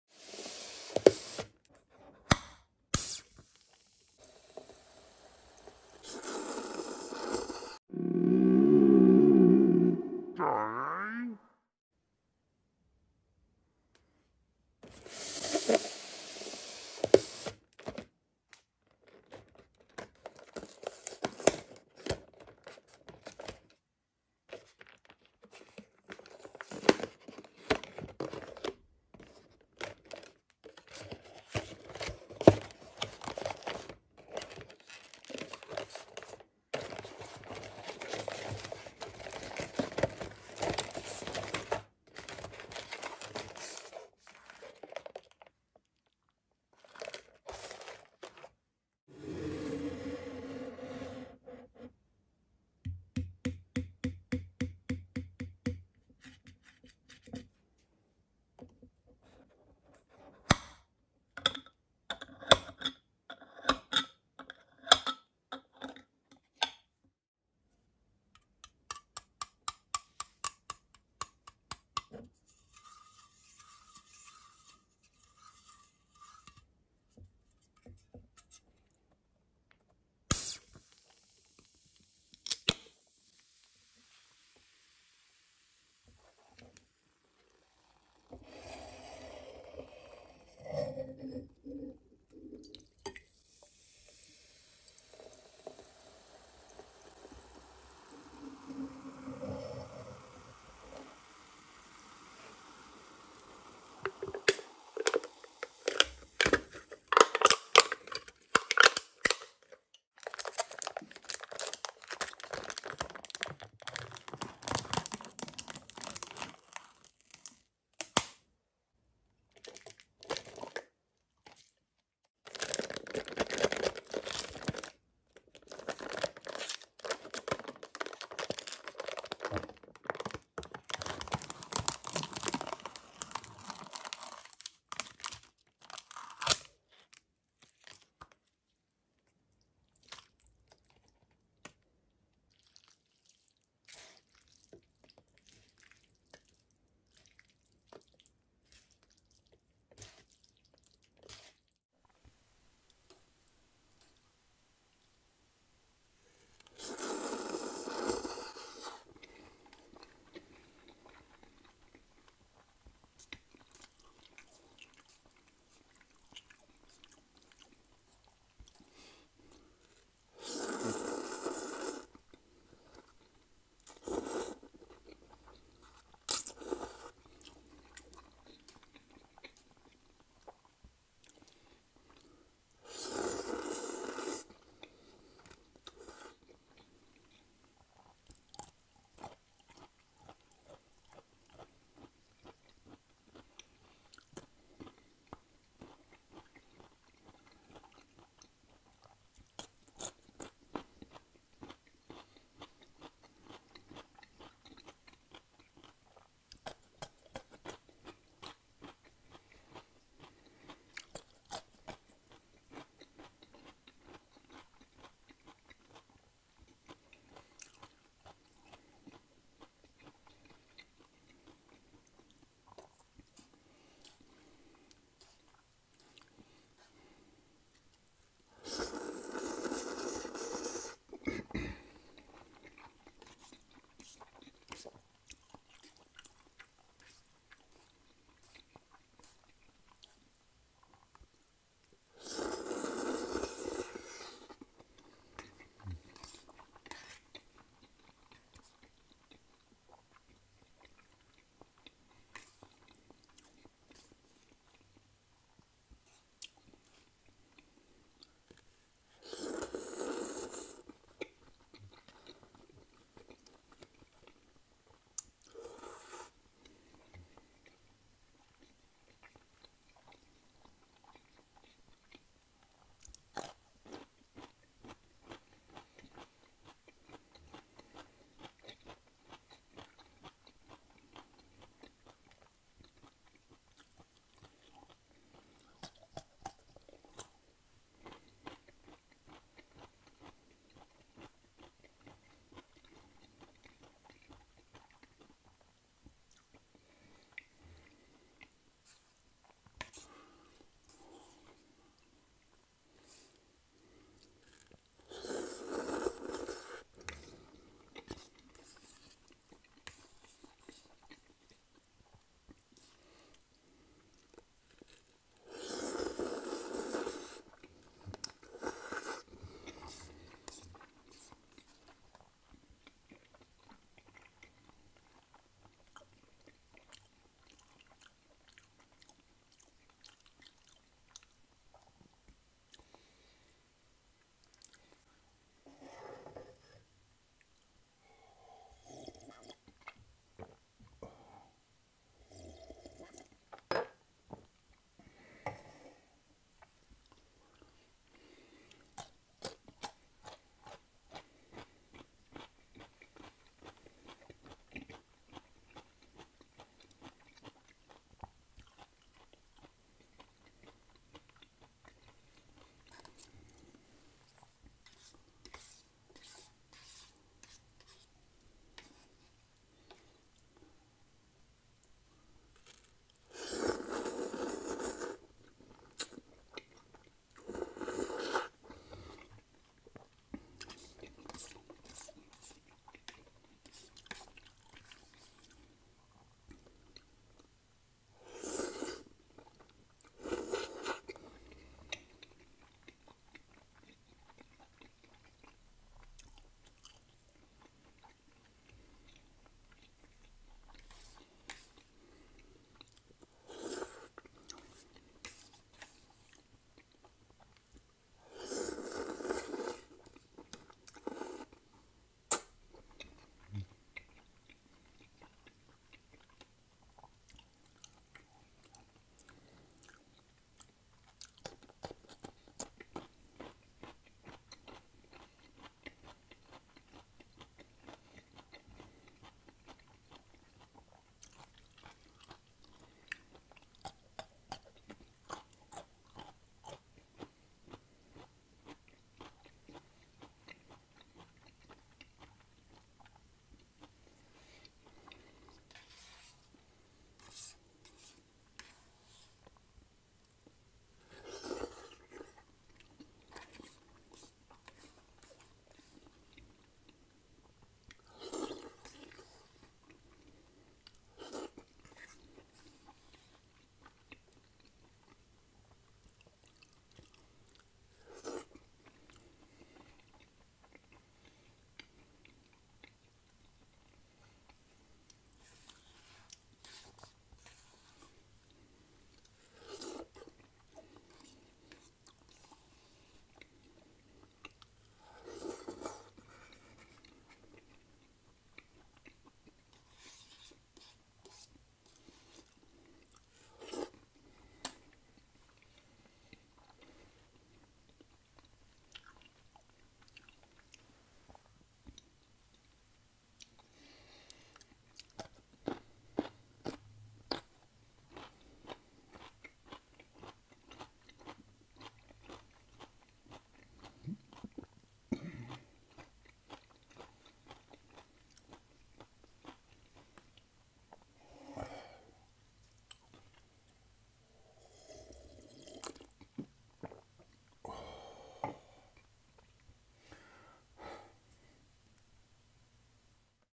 ASMR Pasta CARBONARA and CUCUMBER MUKBANG No talking SOUNDS OF COOKING AND FOOD | Joker ASMR
This video contains the ASMR sounds of eating food. To experience the beauty of ASMR sounds, PLEASE put on your HEADPHONES or mute extraneous sounds !!! Happy viewing !!!